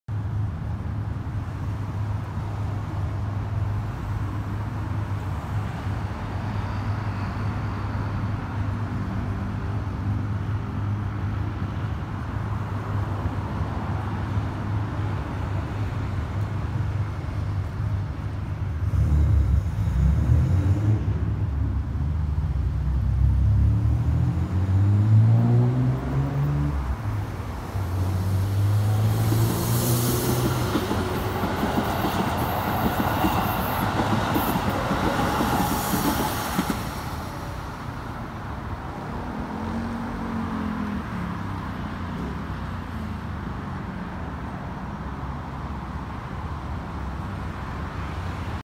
京急線下り6両編成19普通浦賀行き1000形607～612編成が、金沢八景駅下り1番線から追浜駅に向けて発車！
京急線金沢八景駅～追浜駅、六浦駅間の国道16号線沿いの歩道橋の上から撮影！
金沢八景駅下り1番線ホームから、下り6両編成19普通浦賀行き1000形607～612編成の電車が、追浜駅に向けて発車！